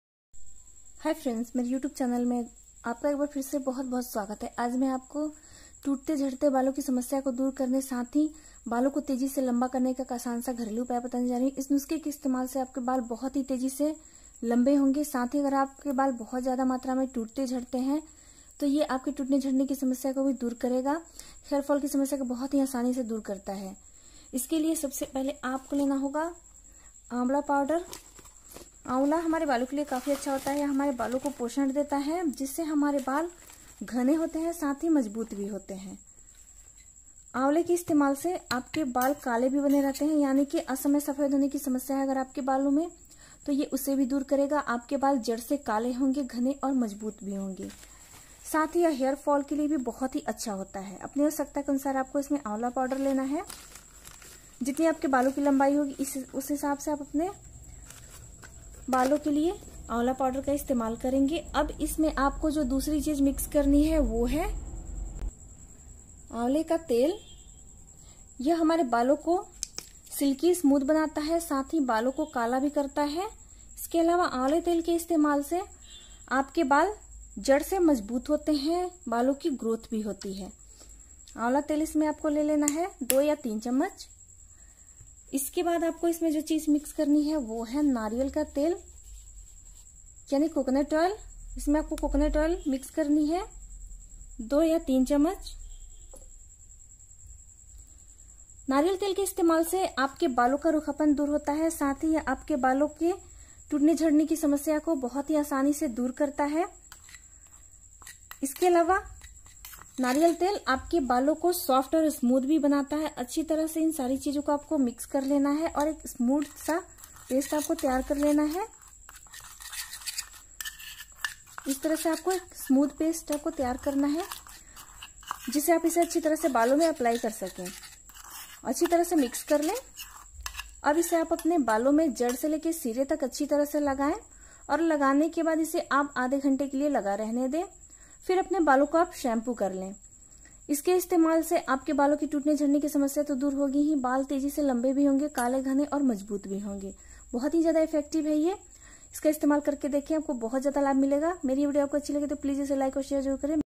ये दो चीजें मिलाकर लगा लो बाल 100 गुना तेजी से लम्बे घने हो जायेंगे! Super Fast Hair Growth
Hair Growth at home
Home remedy for hair Growth

Hair care